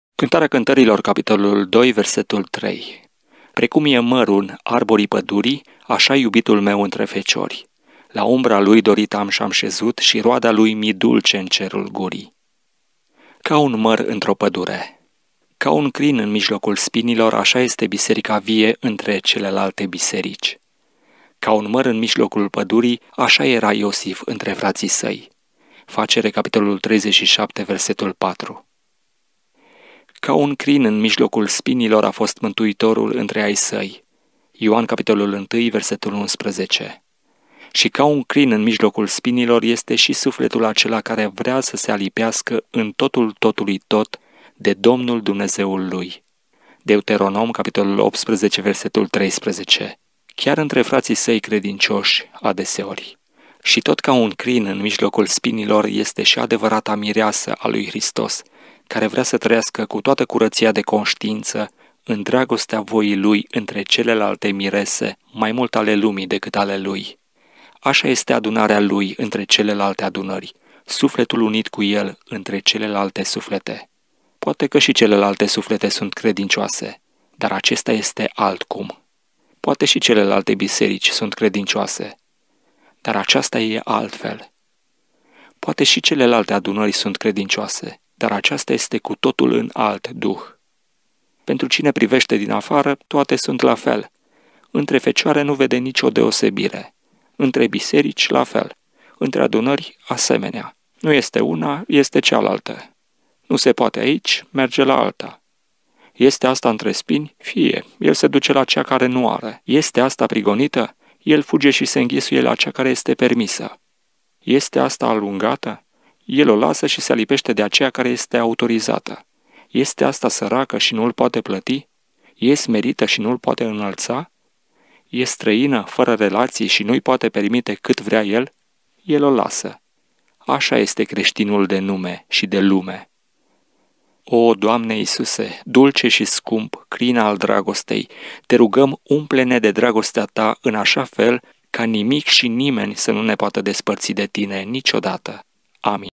Traian Dorz - Ca un măr într-o pădure... (meditație)
Ascultă meditații zilnice din cartea audio „Hristos - pâinea noastră zilnică ”

Capitolul 327 din 22 Noiembrie